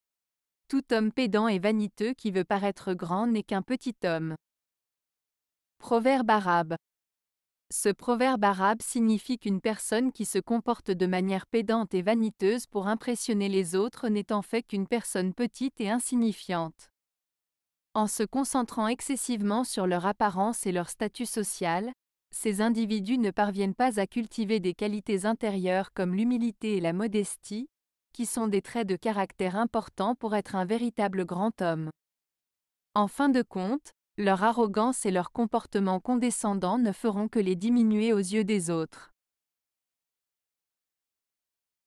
Tout homme pédant et vaniteux qui veut paraître grand n'est qu'un petit homme. - Proverbe Arabe
Ce proverbe arabe signifie qu'une personne qui se comporte de manière pédante et vaniteuse pour impressionner les autres n'est en fait qu'une personne petite et insignifiante. En se concentrant excessivement sur leur apparence et leur statut social, ces individus ne parviennent pas à cultiver des qualités intérieures comme l'humilité et la modestie, qui sont des traits de caractère importants pour être un véritable grand homme. En fin de compte, leur arrogance et leur comportement condescendant ne feront que les diminuer aux yeux des autres.